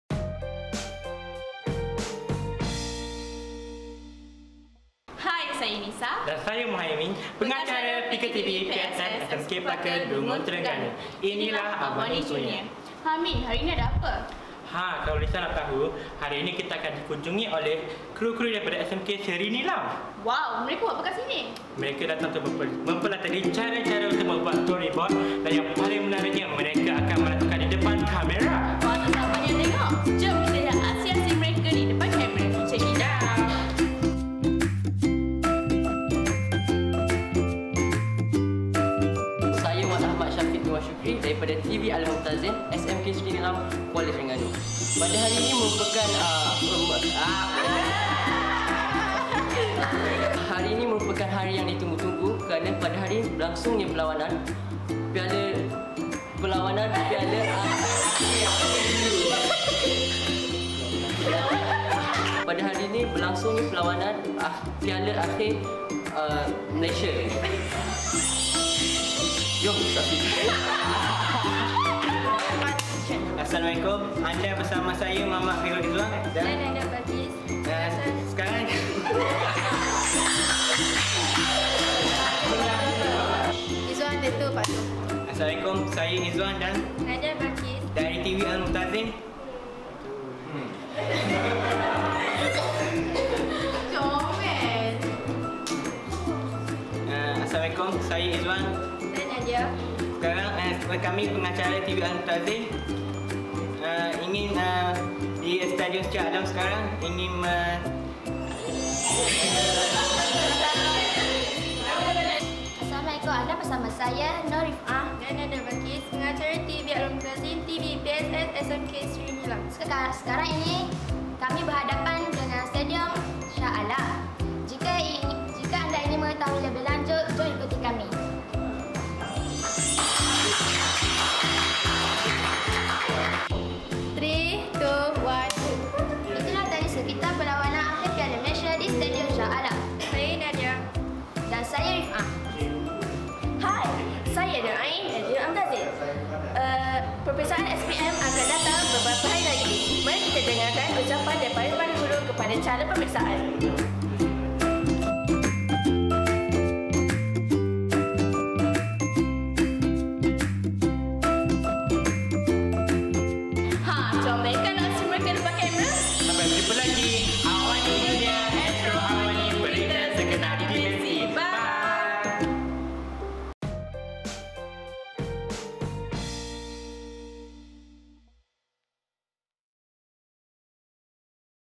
AWANIJr: 3 2 1 Action
Kru TV Al-Mumtazin, SMK Seri Nilam, Kuala Terengganu telah berkunjung ke Studio PK TV PSS SMK Paka untuk melihat dan belajar bagaimana kru PK TV beraksi di depan kamera dengan menggunakan teknik layar hijau.